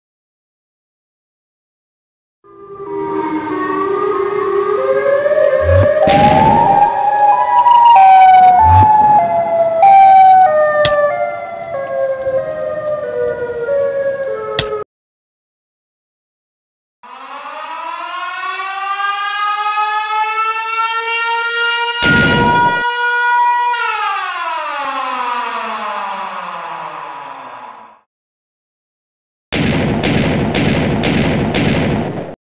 siren head vs enderman
Siren Head is a creation of artist Trevor Henderson. He's a tall creature whose head is a pole with two speakers attached. It's some pretty scary stuff! The Siren Head sound can be creepy for some people so beware!